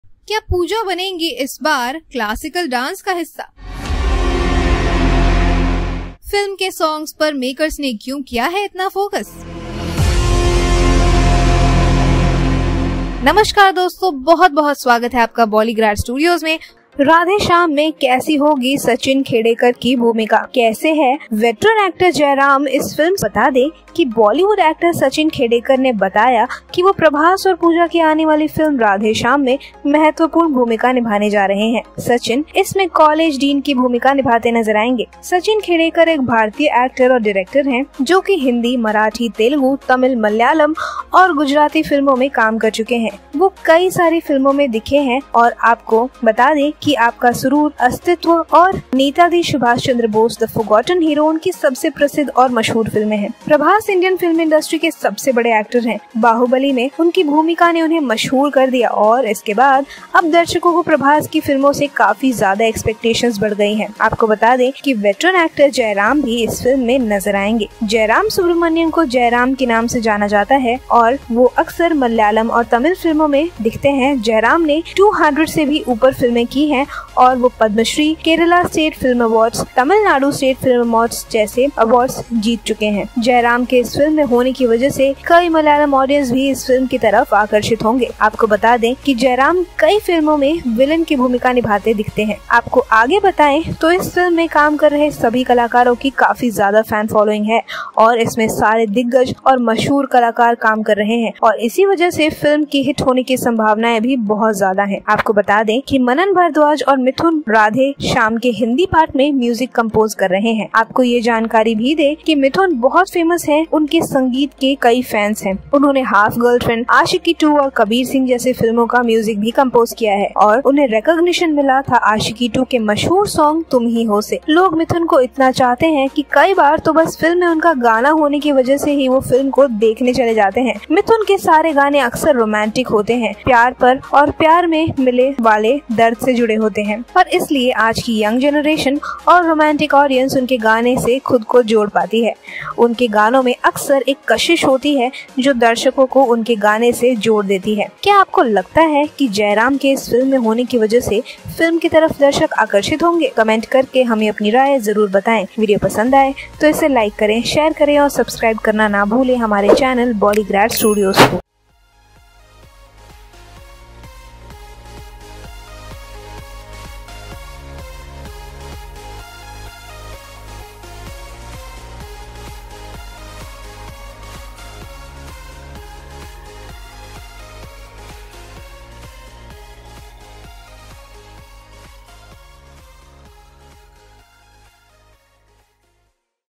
Radhe Shyam | 31 Amazing Facts | Prabhas | Pooja Hegde | KK Radha Krishna Kumar | UV Creations
Here are some Interesting and Amazing Facts about the Movie Radhe Shyam In Which Prabhas and Pooja Hegde are playing lead Role and is Directed by Radha Krishna Kumar.

0:00 Intro
0:25 What will be the Role of Sachin Khedekar in the Movie "Radhe Shyam"?
1:05 What are Some Fantastic Facts About Jairam Subramanyam?
1:45 Who is Handling the Hindi Music Department in the Movie "Radhe Shyam"?

Radhe Shyam is a romantic entertainer movie directed by KK Radha Krishna and produced by Gopi Krishna movies in association with UV creations banner. The movie cast includes Prabhas and Pooja Hegde are in the main lead roles.

Buzz has it that Pooja has a twin sister and both the girls fall in love with Prabhas without knowledge of each other. In the end, one of them will sacrifice their love and life to another one. The story seems really interesting but we don’t know yet how far this story is true. An official confirmation regarding the same is awaited.

In the latest development, Marathi actor Sachin Khedekar, who's also worked in several Hindi movies and was also seen in Allu Arjun's Ala Vaikunthapuramuloo, has joined the crew. The actor is said to be playing the role of a doctor in the movie.

Sachin Khedekar is believed to have revealed the real plot of Prabhas movie Radhe Shyam. As per the actor, the movie deals with the conflict between astrology science.

Credits:
Editor: Jeet Saha
Anchor: Pankhuri
Script: Diksha Rajput